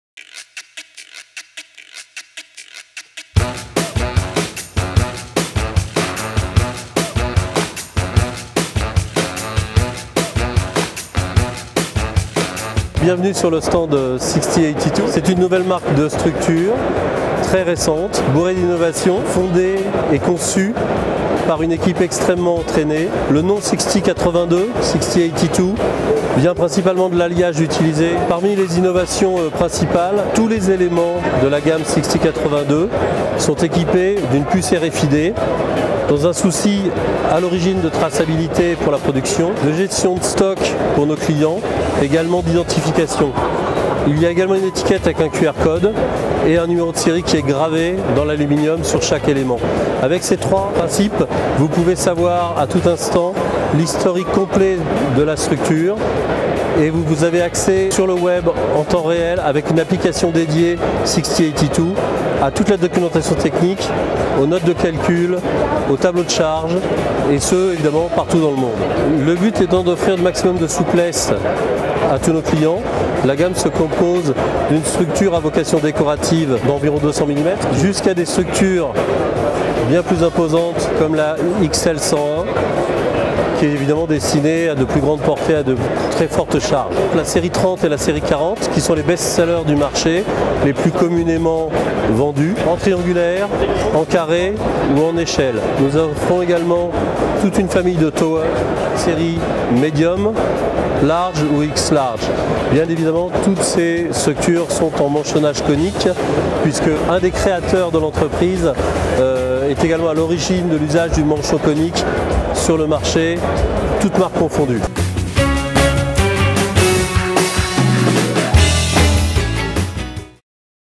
LAUNCH 2018 // Sixty82, the new truss 2.0 manufacturer
[EN]
We meet Alain René Lantelme (Deputy General Manager of Axente) at Prolight + Sound for the tour of the stand as well as a demo of the brand new application for smartphone.
As a reminder: Sixty82 is a brand of aluminum structures launched very recently in March 2018 by three groups of experts in the field, English, French and Dutch.
[FR]
Nous retrouvons Alain René Lantelme (directeur général adjoint d’Axente) à Prolight+Sound pour le tour du stand ainsi qu’une démo de la toute nouvelle application pour smartphone.
Pour rappel : Sixty82, c’est une marque de structures en aluminium lancée très récemment en mars 2018 par trois groupes d’experts en la matière, anglais, français et hollandais.